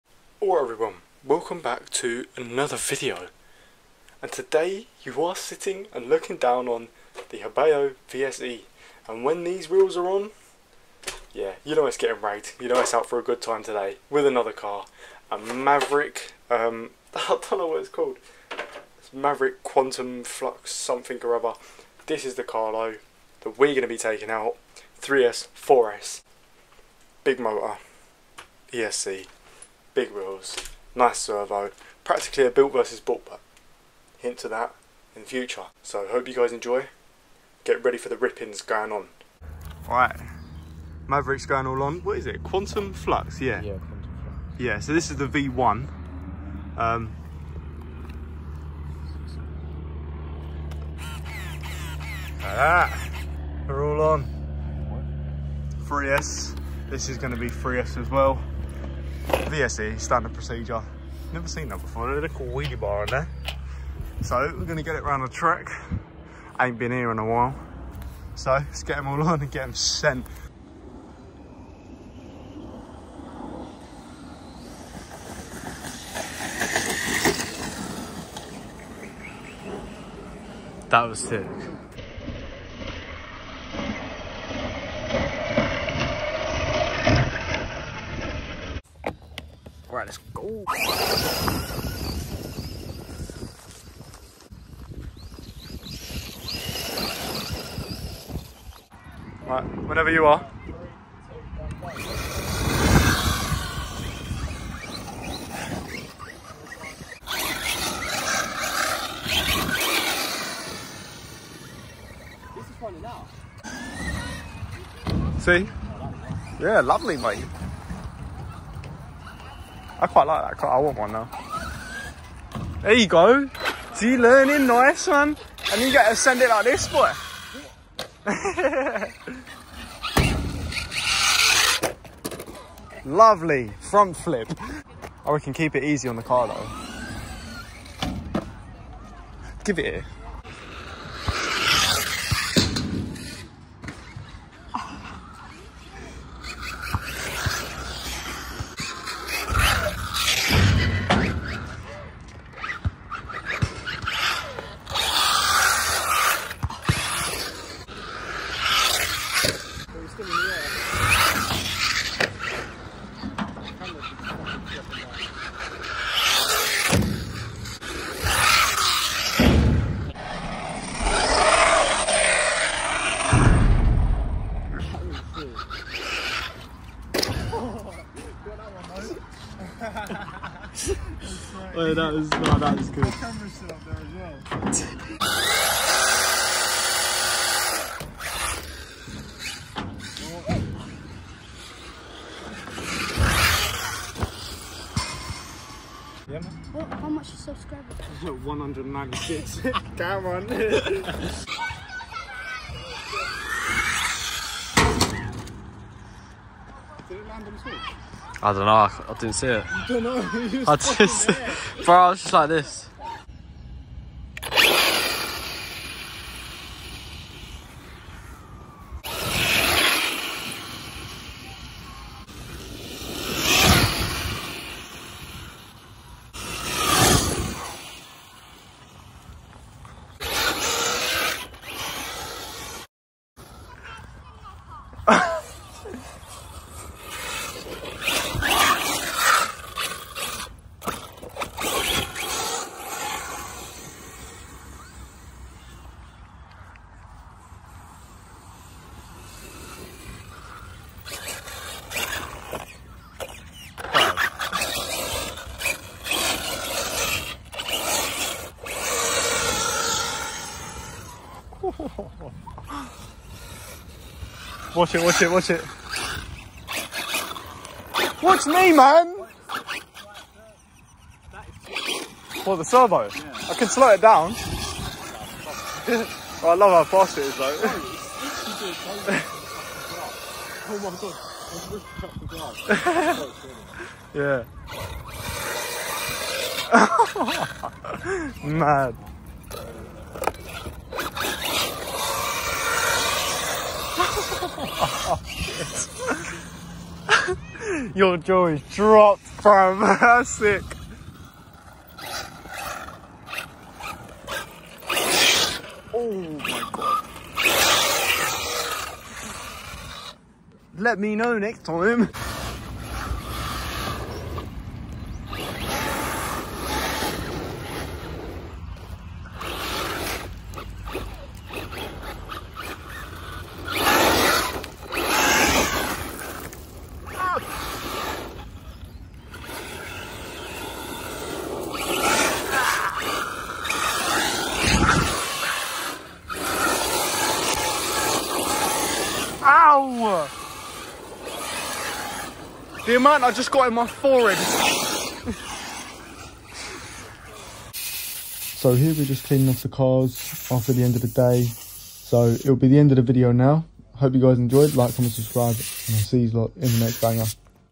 These cars are INSANE !!
hyper vs and maverick quantum flux go shoulder to shoulder at the forest and the field for an action filled adventure with lots of learning and prep for the future and as per usual, like and subscribe, always remember the good old magic words, SEND IT BOYYY !!!

Chapters:

0:00 - 0:38 - intro
0:39 - 1:10 - turning on
1:11 - 5:11 - ripping
5:12 - 5:46 - gravel drifting
5:47 - 5:57 - outro